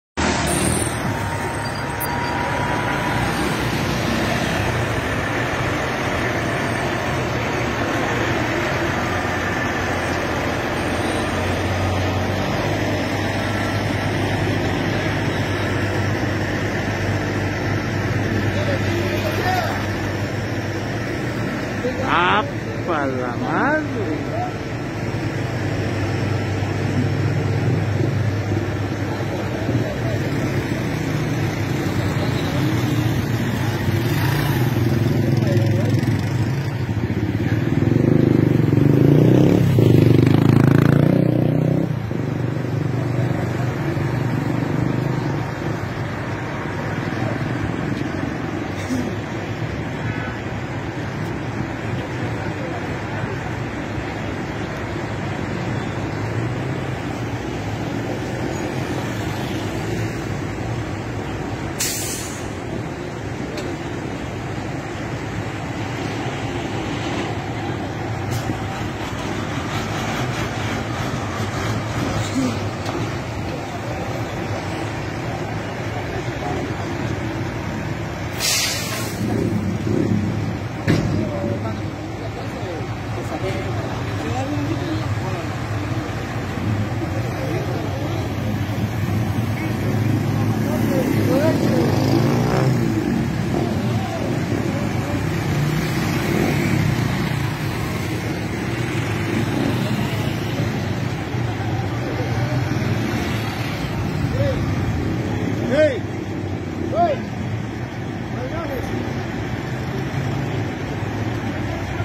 PRECAUCIÓN EN CARRETERA CA2.
Vehículo con carga extra larga circula por la carretera CA2, derivado de esto, la circulación de vehículos es lenta y limitasa.